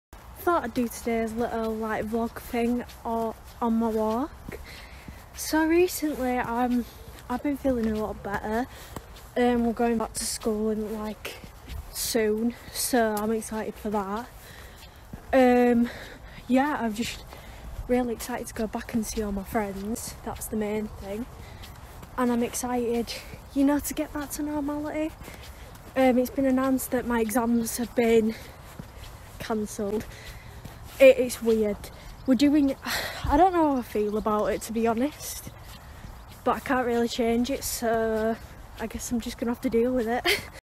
Light At The End Of The Tunnel
Ellie is happy that things are beginning to go back to normal.